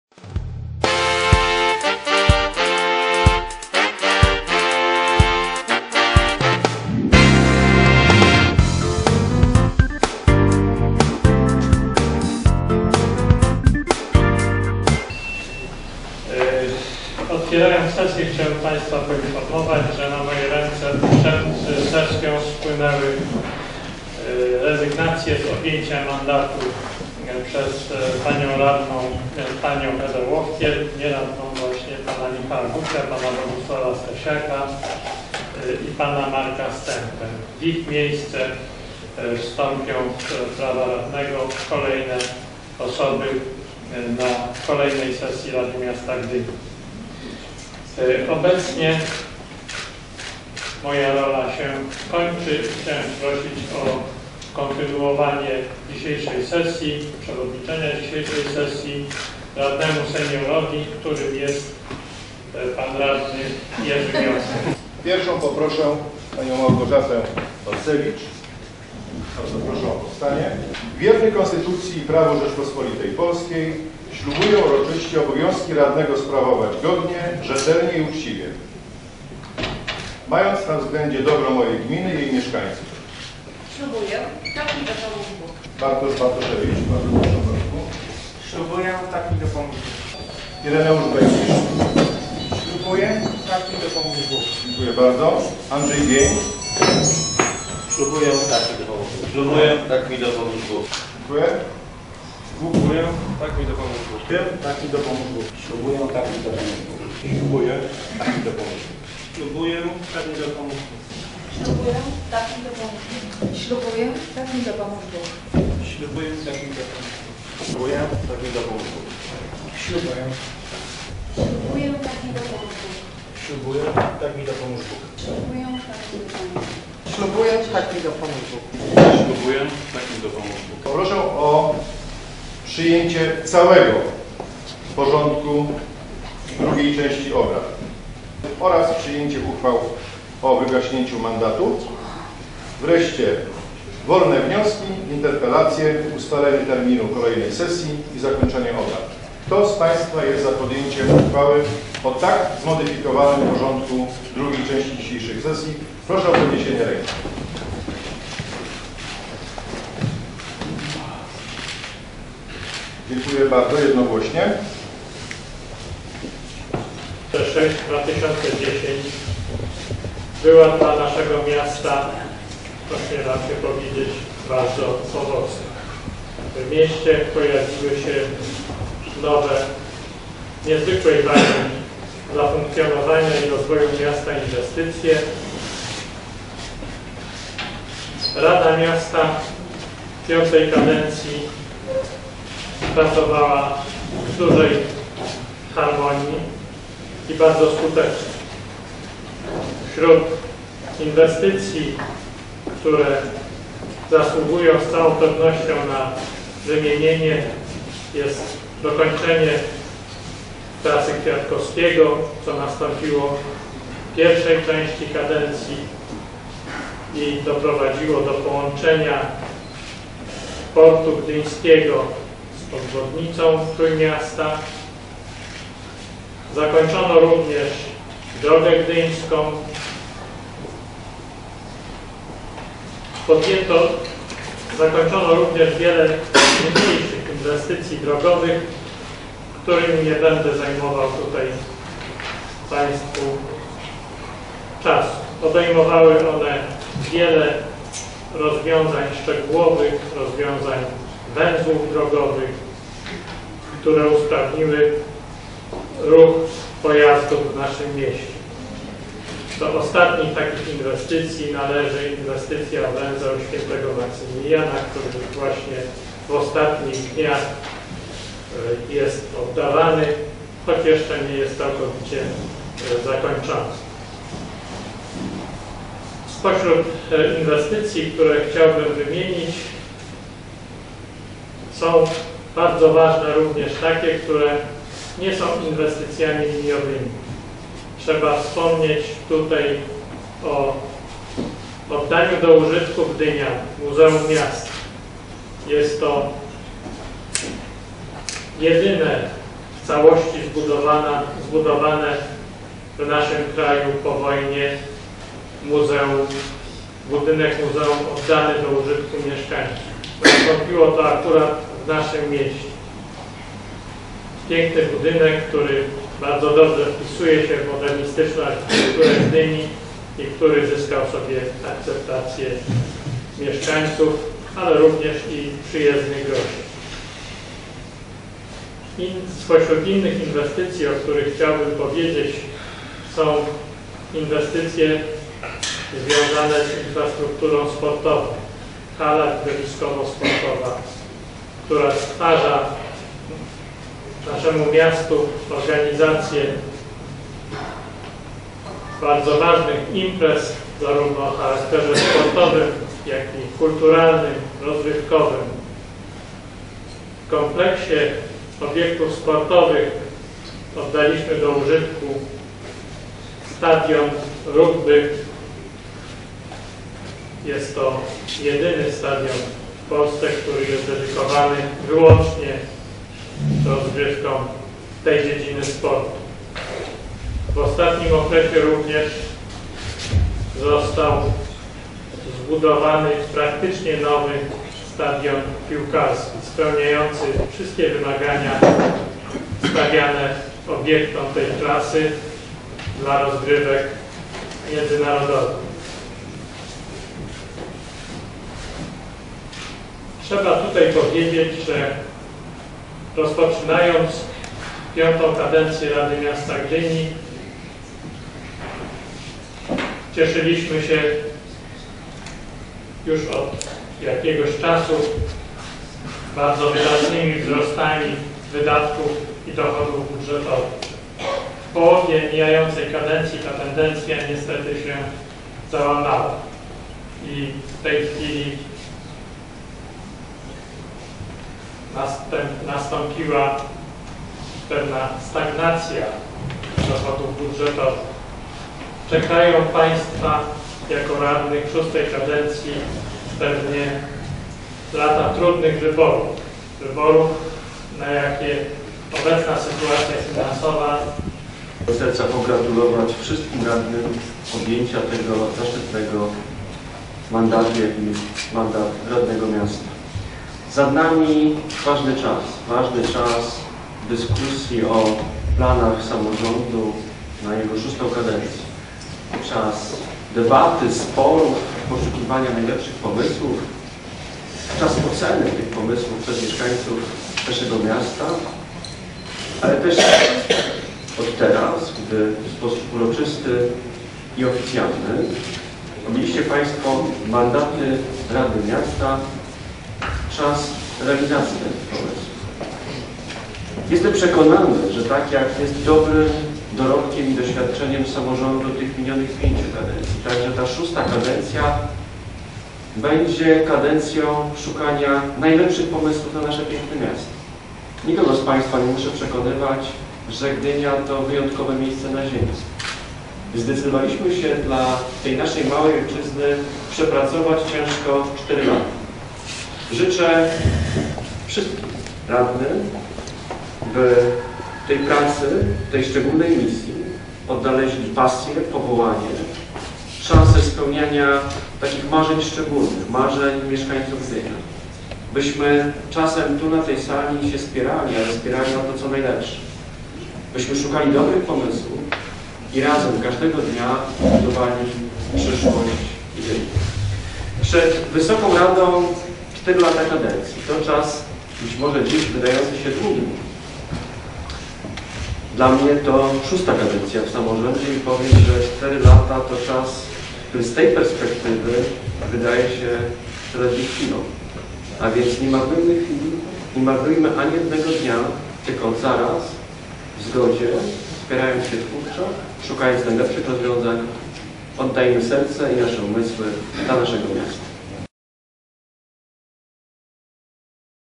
GDYNIA I sesja VI kadencji
30 listopada 2010 odbyła się I sesja VI kadencji Rady Miasta Gdyni. Rajcy złożyli uroczyste ślubowanie i wybrali Joannę Zielińską na stanowisko przewodniczącej.